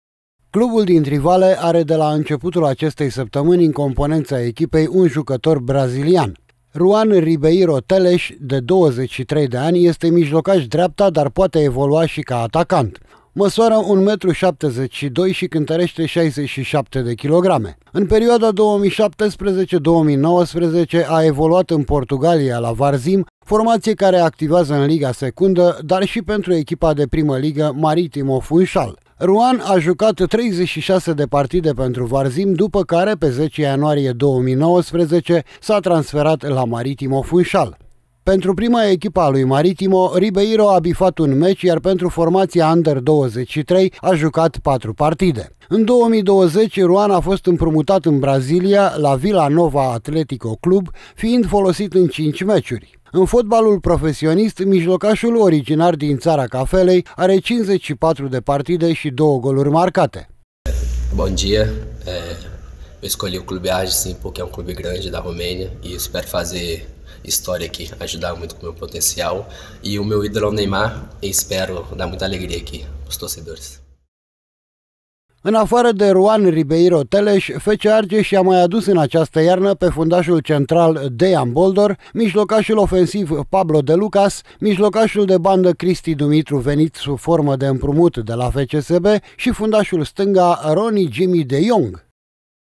Neymar de Trivale alias Ruan Ribeiro Teles ( 09 02 2021 )
Scurtă descriere:
 F.C. Argeş a anunţat pe site-ul oficial perfectarea unui nou transfer. Echipa din Trivale a ajuns la un acord pentru un sezon şi jumătate cu brazilianul Ruan Ribeiro Teles. Acesta se pregăteşte de câteva săptămâni cu alb-violeţii, iar de luni a devenit oficial jucătorul lui F.C. Argeş.